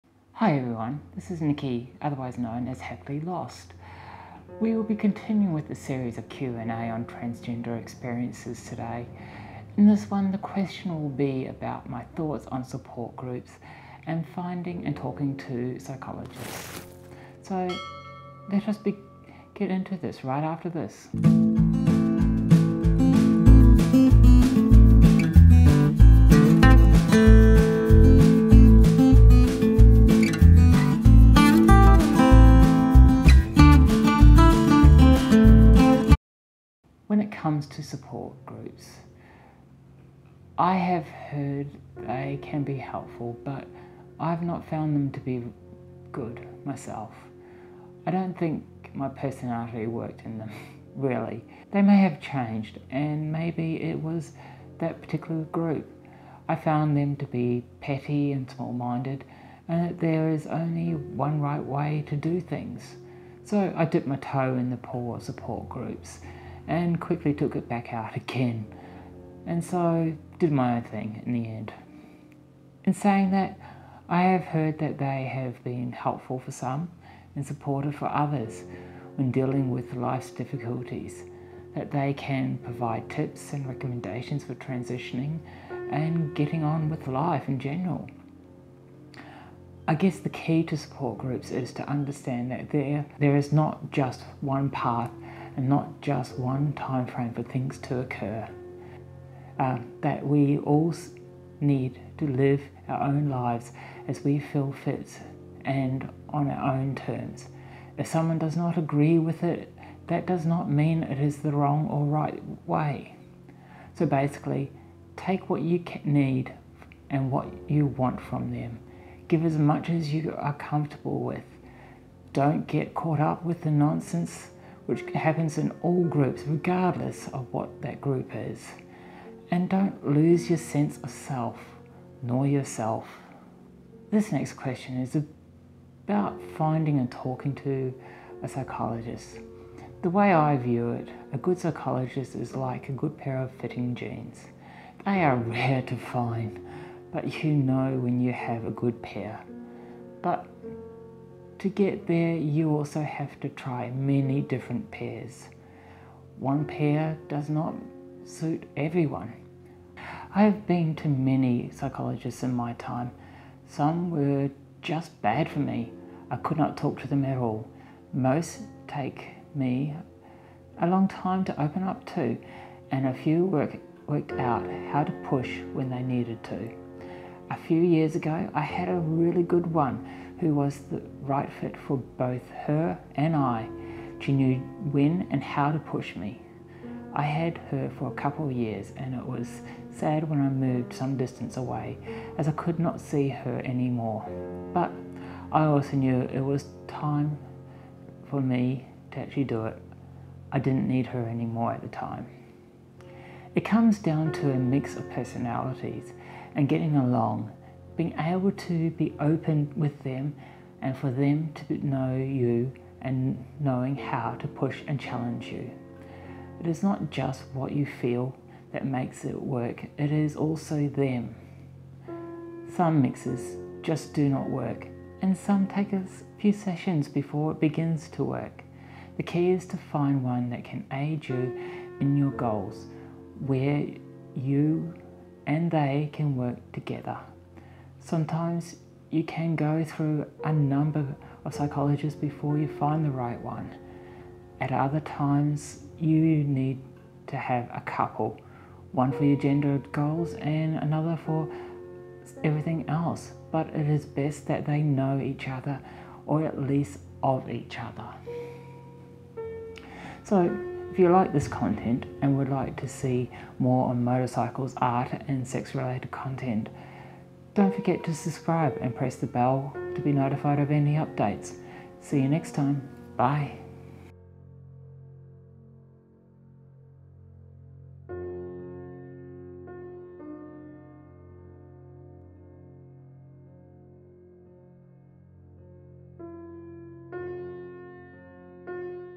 Q and A with a trans woman Part VI of VII
Hi everyone,

With one more part after this one we continue with our current series of Q and A with a trans woman. Relating to my experiences of being a trans woman transgenderexperiences transexperiences transgender and my thoughts on trans issues transgenderissues transissues. Some of these issues many of us deal with regardless of gender identity. Anyway, I hope you enjoy this series, more will be release in the following weeks.

So put your feet up and enjoy everyone from Happily Lost

Happily Lost